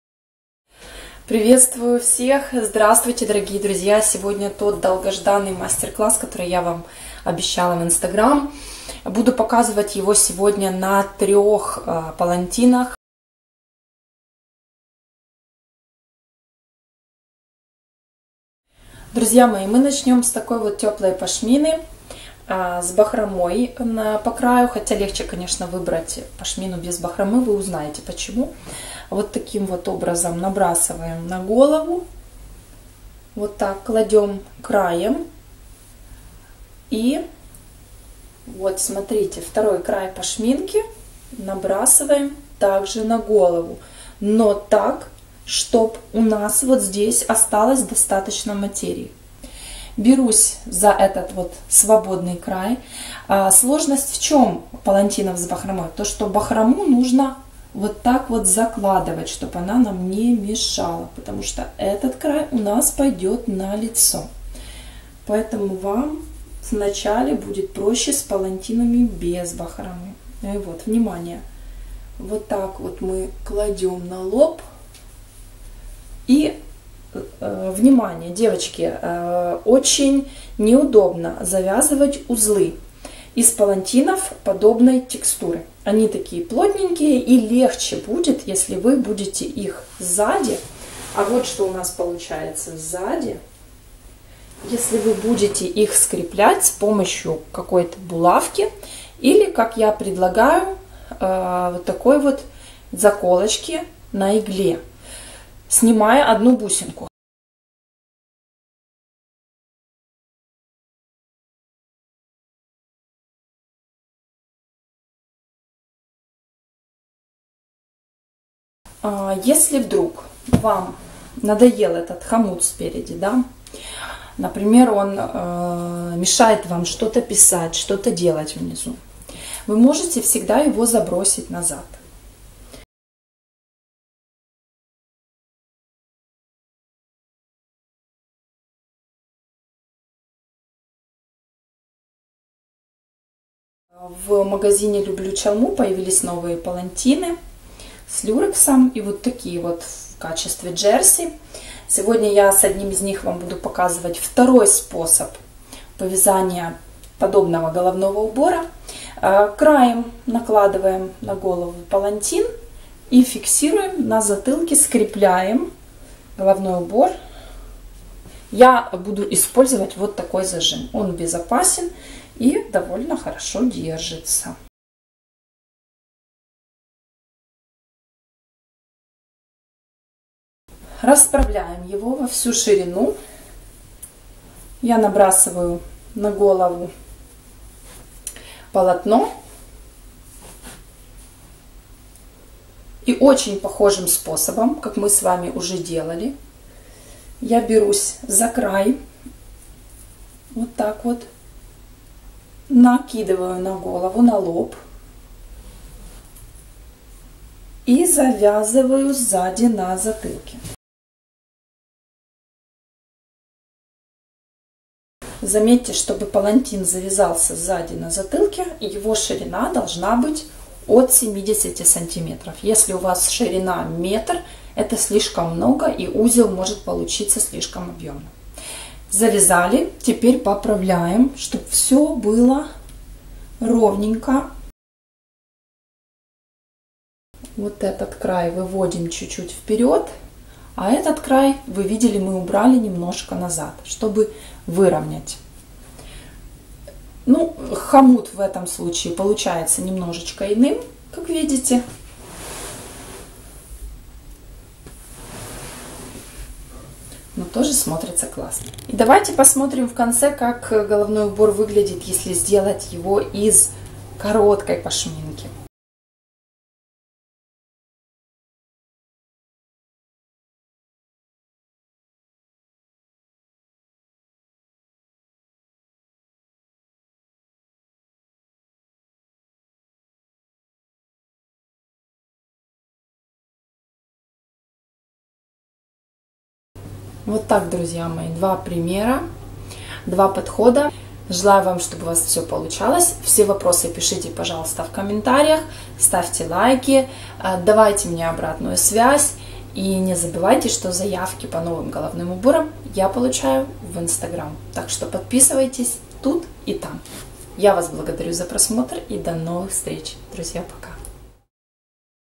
2 способа для пашмины и палантина - как повязать головной убор С ХОМУТОМ.
Моя страничка в Инстаграм - @annadiva_youtube
Магазин палантинов и аксессуаров к ним (Украина) - @lublu_chalmu

Размеры пашмин в видео:
1) Малиновая (акрил) - 180 х 60 см.
2) Пятнистая (джерси) - 180 х 85 см.
3) Серая (хлопок+люрекс) - 170 х 68 см.